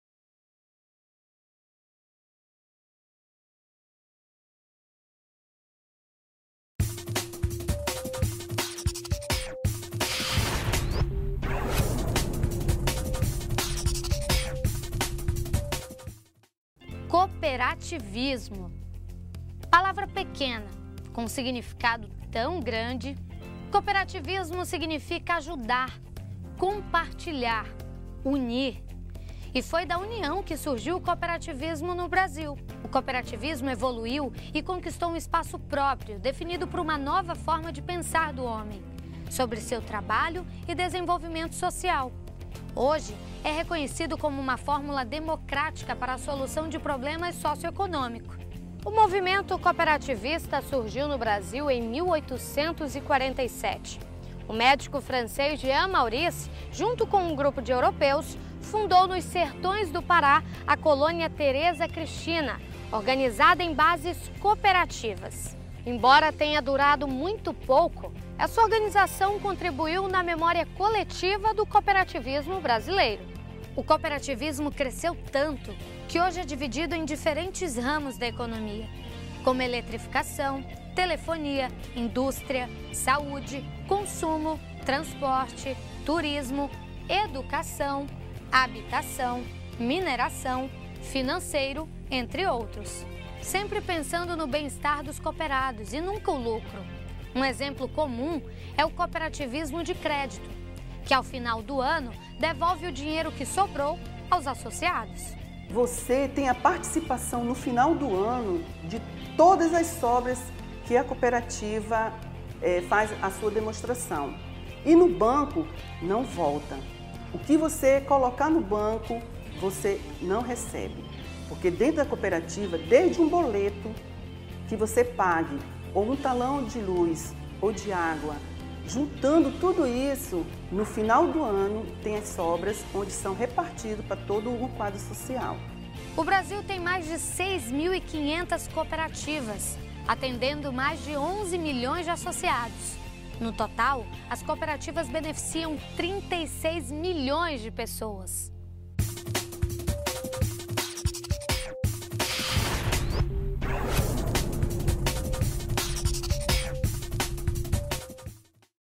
Cooperativismo no Brasil
Vídeo de apoio ao curso Introdução ao Cooperativismo do Instituto Federal de Rondônia.